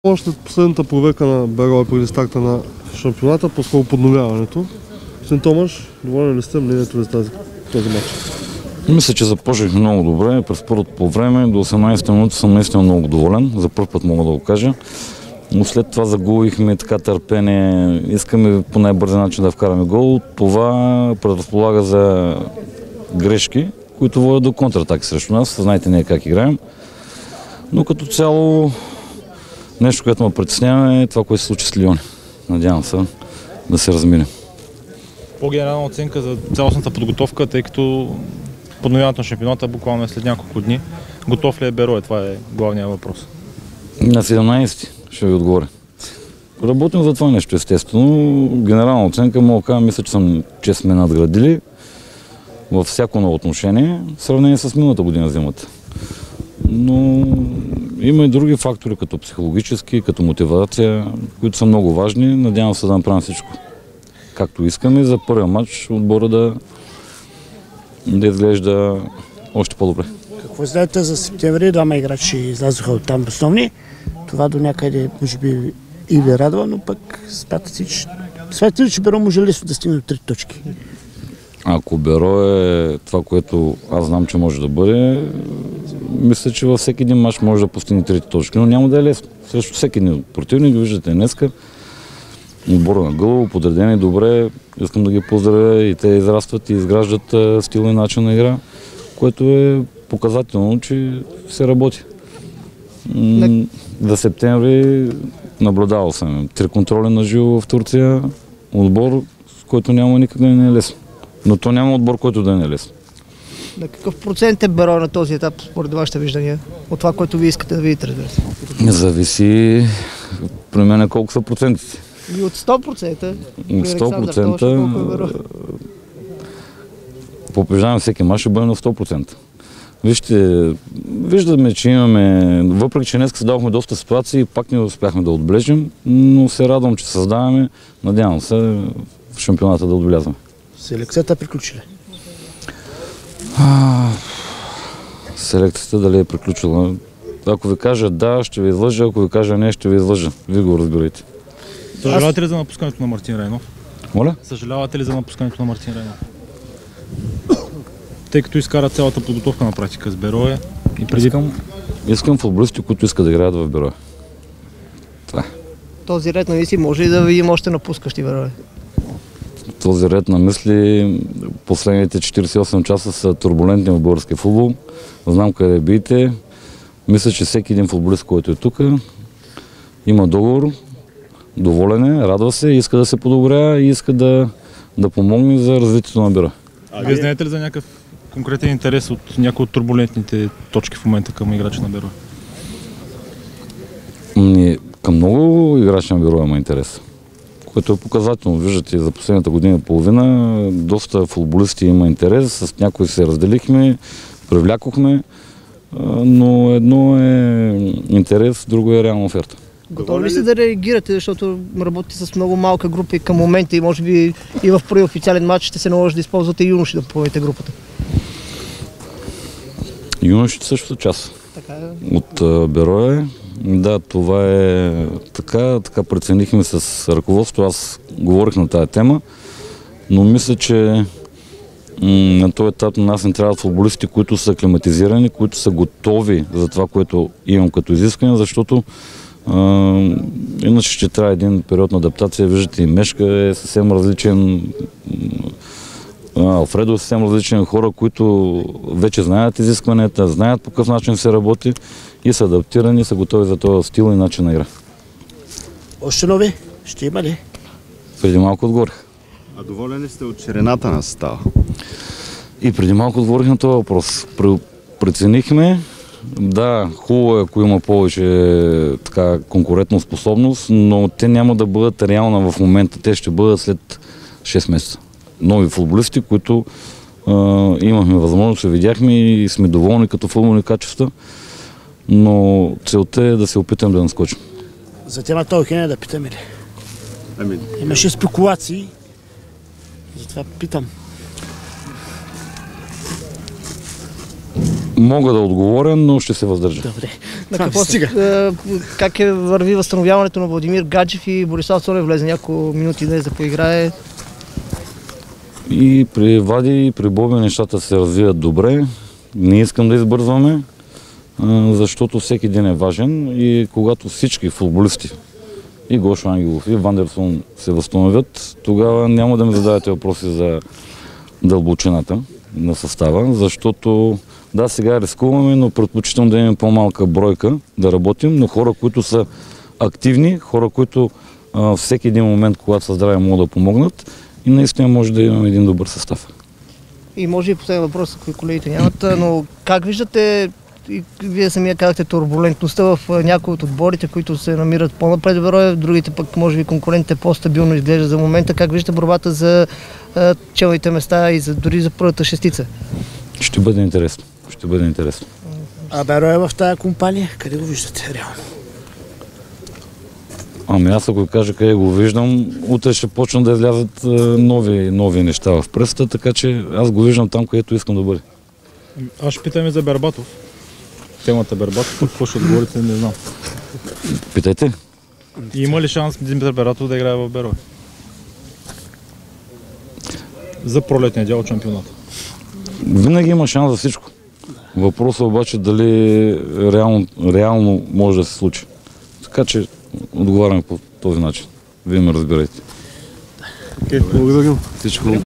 Александър Томаш след контролата с "Ботев" (Гълъбово)
Мнението на наставника след последната проверка от зимната подготовка